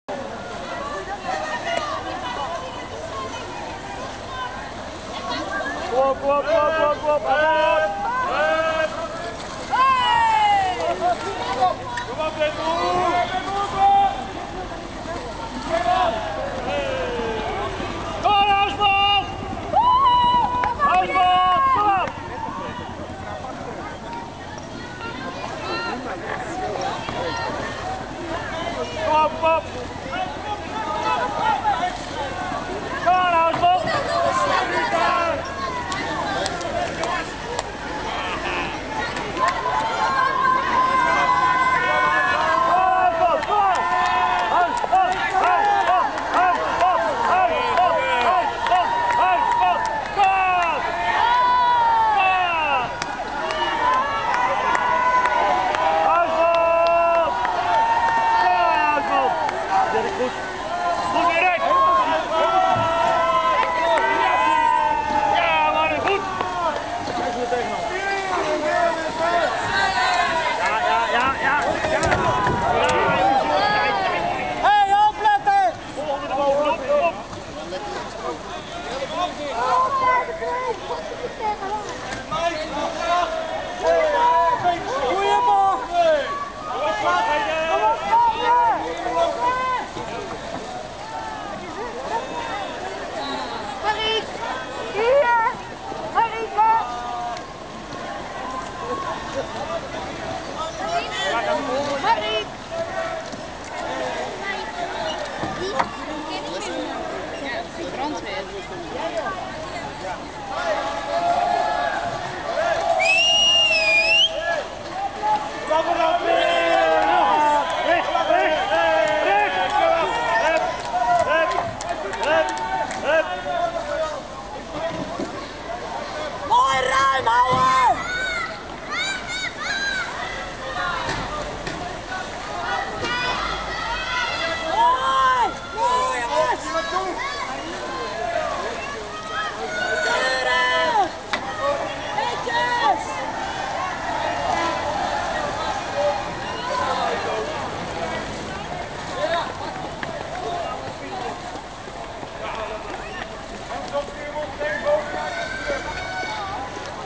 Grachtenrace 2007
The annual canal race. Location: Keizersgracht/Leidsegracht.